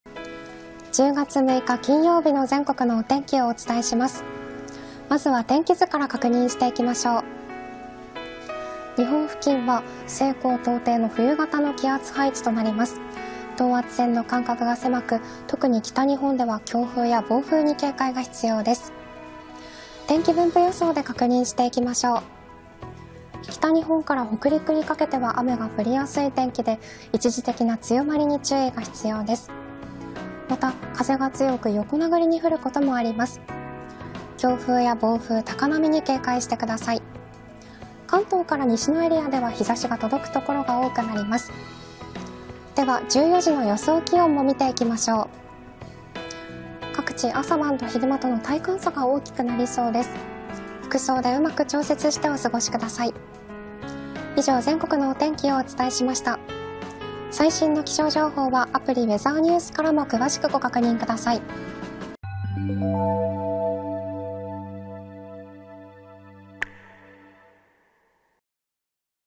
【10月6日(金)】北日本は荒天警戒 晴れる関東以西も風は強め 寒暖差あるので服装で調整を
低気圧が発達し、北日本を中心に荒天となります。特に暴風に警戒をしてください。晴れる関東以西の太平洋側も北寄りの風が強めに吹く見込みです。